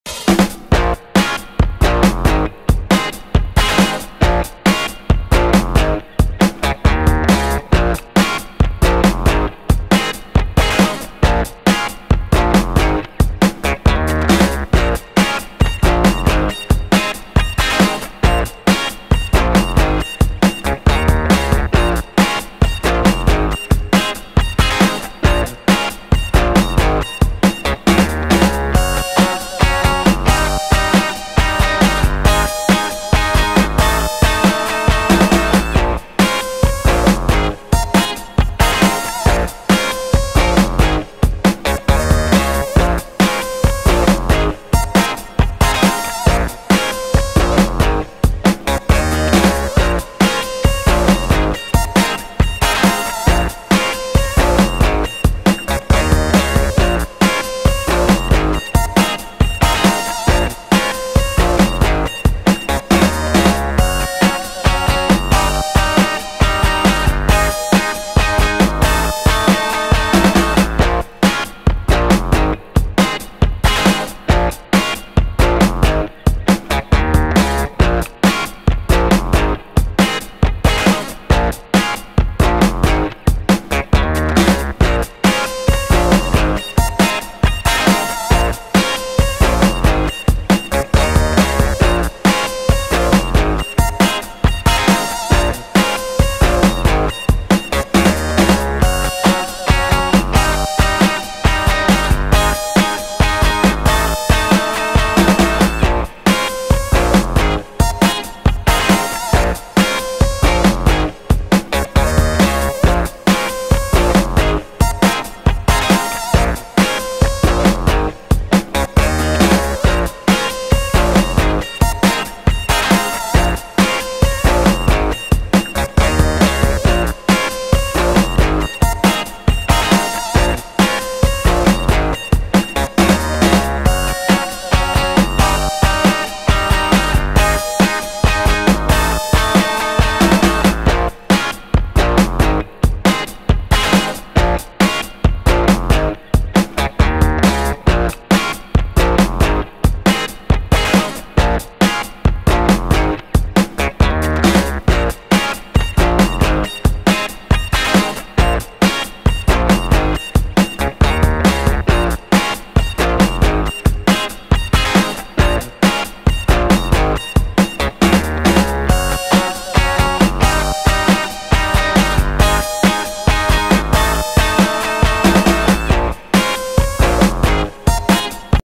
Crazy Off Road Buggy Driver
Crazy off road buggy driver, driving around the bank walls of a dam.
Sliding, Drifting, Donuts, Jumping and getting muddy!!!